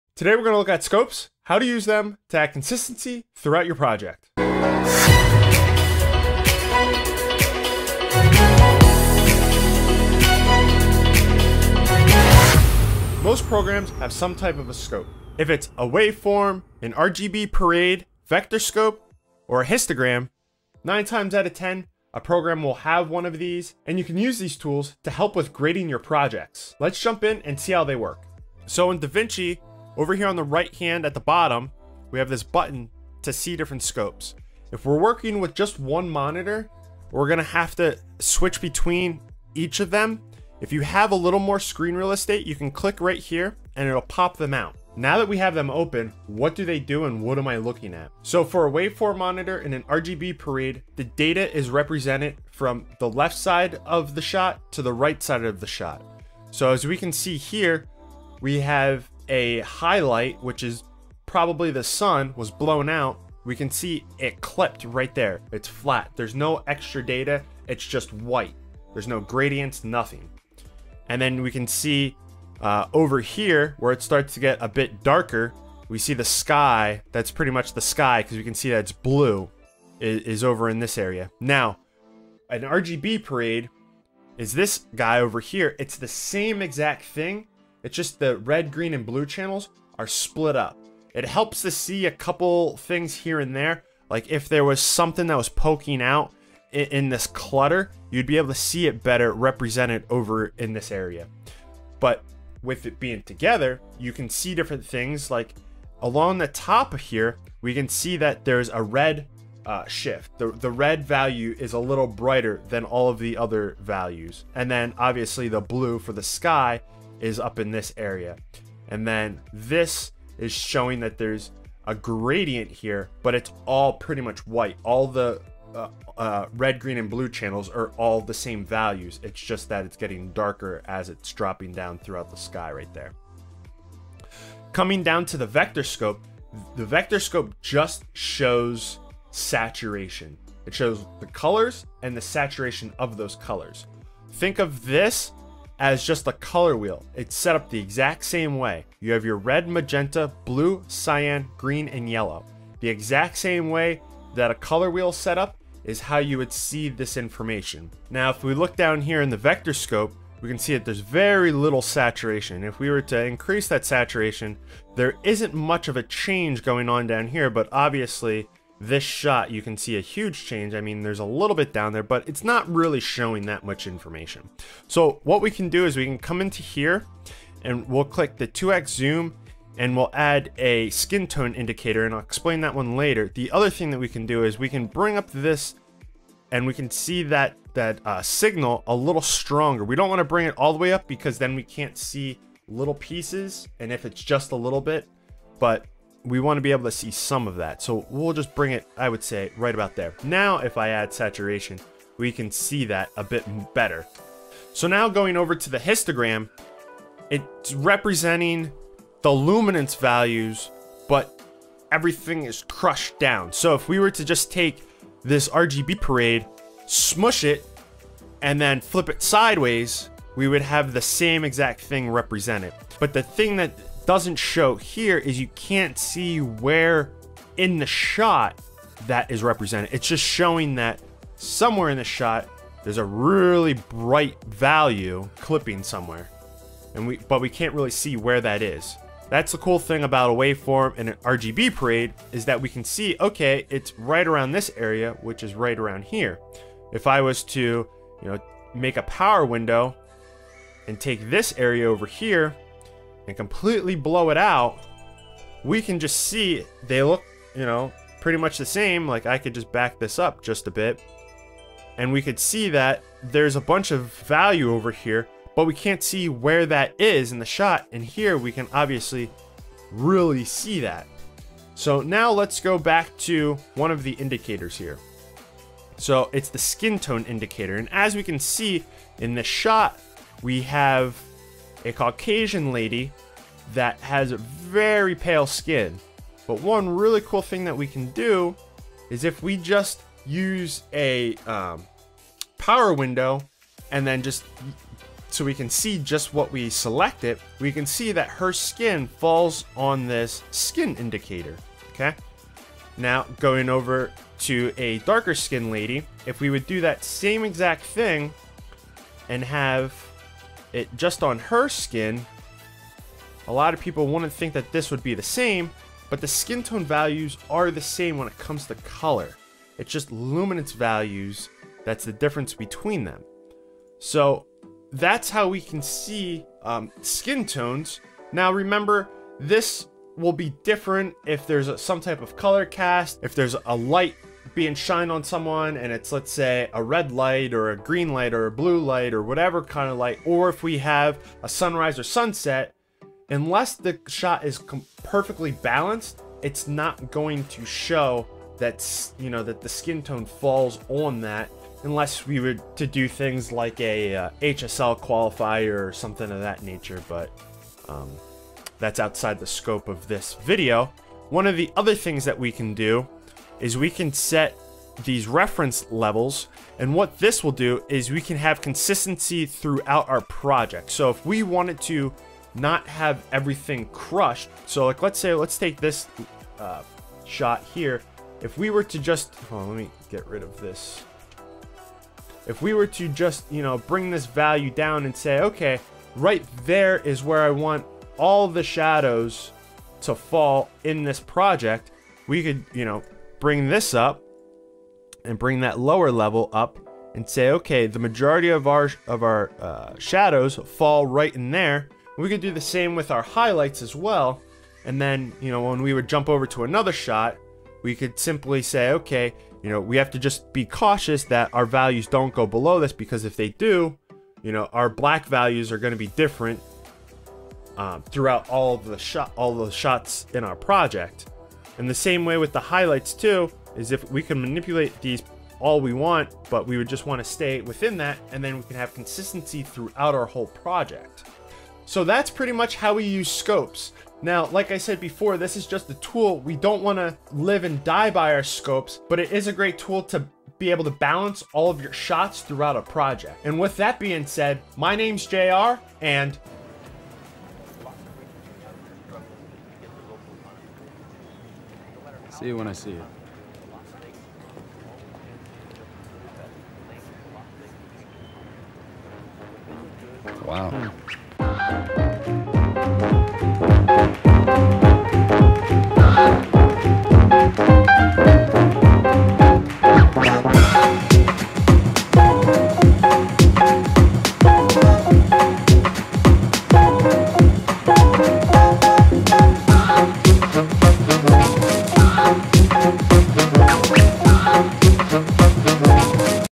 How to use scopes for color grading (DaVinci Resolve)
Quick guide on how scopes work.